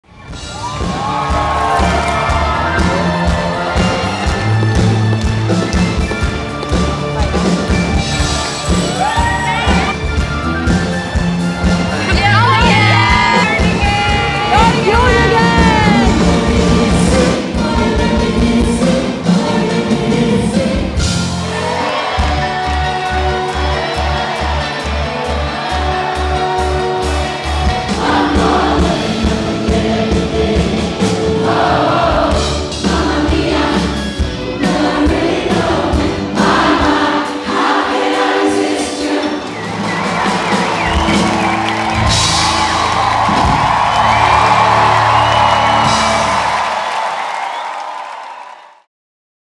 Dubai Opera - Bjorn Again Night at Dubai Opera
Throwing it back to an internationally acclaimed show BJÖRN AGAIN last night at Dubai Opera l🌟 The satirical and light-hearted ABBA tribute had the audience singing and enjoying timeless hits that defined an era.